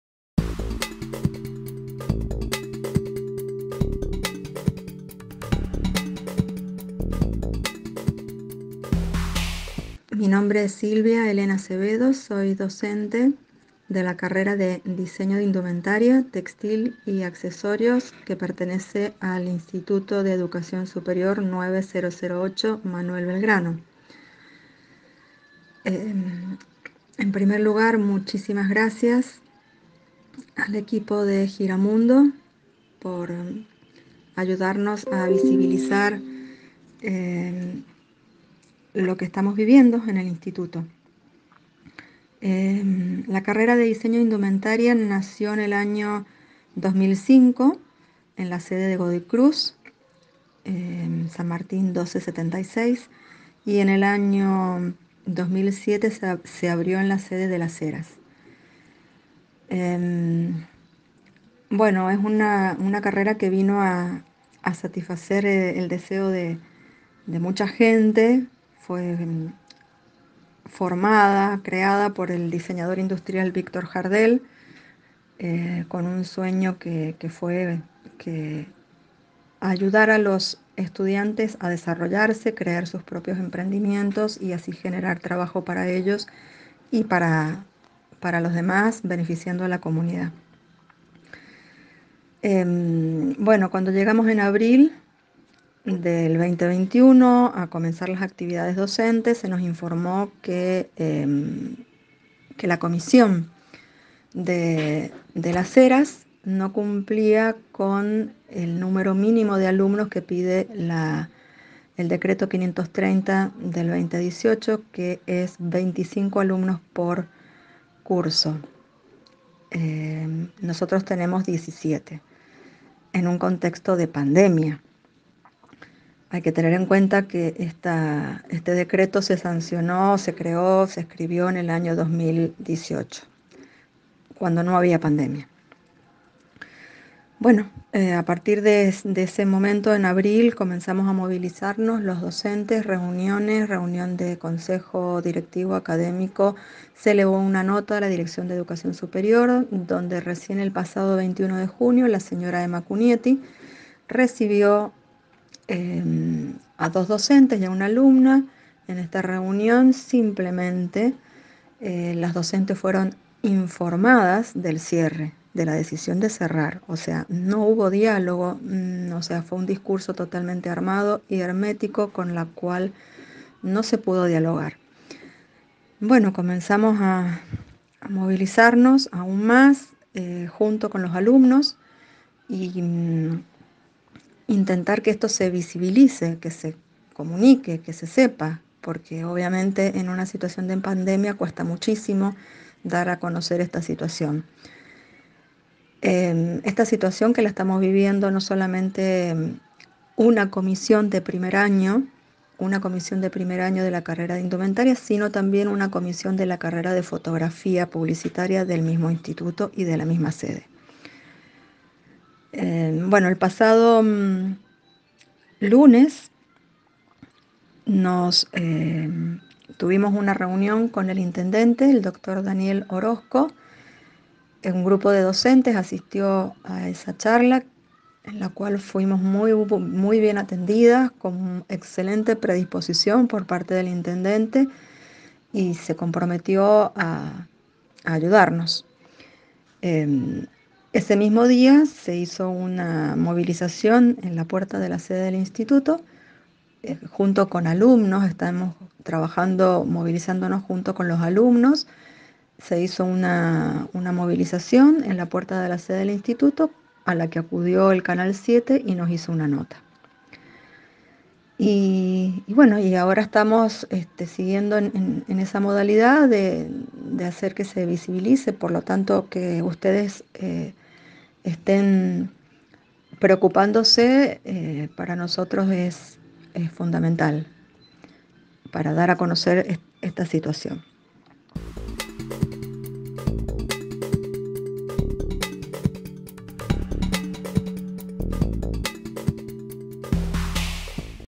Dirección de Educación Superior quiere cerrar el IES 9-008 "Manuel Belgrano" de Las Heras
La Dirección de Educación Superior de la provincia de Mendoza pretende cerrar el I.E.S. 9-008 “Manuel Belgrano” de Las Heras.
Silvia Elena Acevedo es Docente en dicho Instituto y nos cuenta de como se viene dando el proceso de lucha por parte de la comunidad educativa (docentes y estudiantes) para intentar sostener el espacio e impedir que se aplique una resolución emitida en 2018 que llevaría al cierre de las comisiones de las carreras de indumentaria y fotografiá.
Desde el mes de abril, que se viene reclamando una solución ante esta incertidumbre y recién en el mes de junio la Directora de Educación Superior reafirmo la decisión de cerrar la Institución, con lo cual se trasladarían docentes y estudiantes al I.E.S. de Godoy Cruz. Por ello, la comunidad educativa viene movilizándose y lograron el compromiso del Intendente de Las Heras Dr. Daniel Orosco para mediar en la situación y lograr una solución favorable a los docentes y estudiantes.
La resolución de 2018 no contempla la situación de pandemia que atravesamos actualmente y por ello, en vez de prever posibles soluciones favorables estaría perjudicando de manera considerable a quienes trabajan y estudian en el I.E.S. Manuel Belgrano de Las Heras.